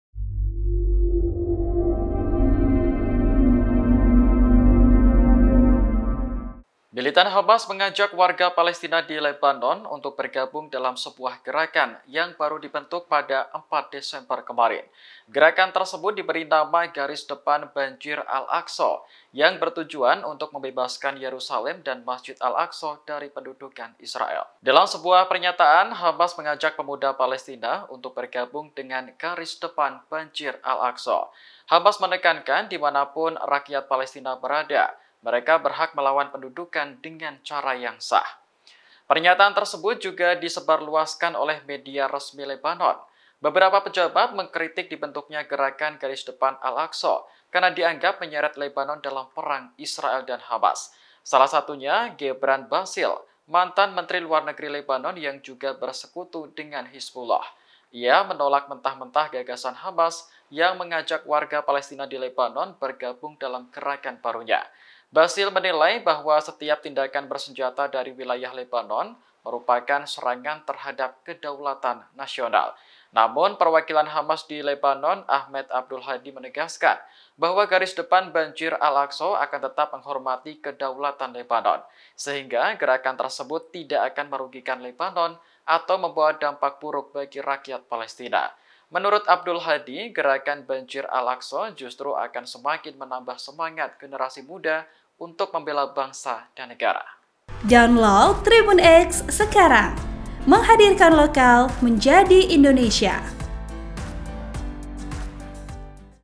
Tujuan Mulia Hamas Bentuk Gerakan Garis Depan 'Banjir Al Aqsa': Bebaskan Yerussalem & Masjid Al Aqsa
TRIBUN-VIDEO.COM - Militan Hamas mengajak warga Palestina di Lebanon untuk bergabung dalam sebuah gerakan yang baru dibentuk pada 4 Desember.

Gerakan tersebut diberi nama "Garis Depan Banjir Al-Aqsa" yang bertujuan untuk membebaskan Yerussalem dan Masjid Al-Aqsa dari pendudukan Israel.

Dalam sebuah pernyataan, Hamas mengajak pemuda Palestina untuk bergabung dengan Garis Depan Banjir Al-Aqsa.

VP: Fegi